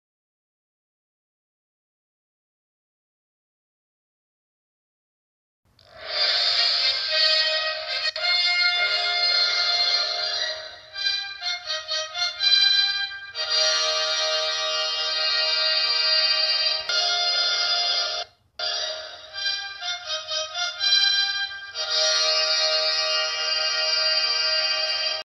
Iniziale e Finale TG Una
Rifacimento sigla di TG Una, fatto dalla RaiSet!!
Direttore Ettore
Regia Lorenzo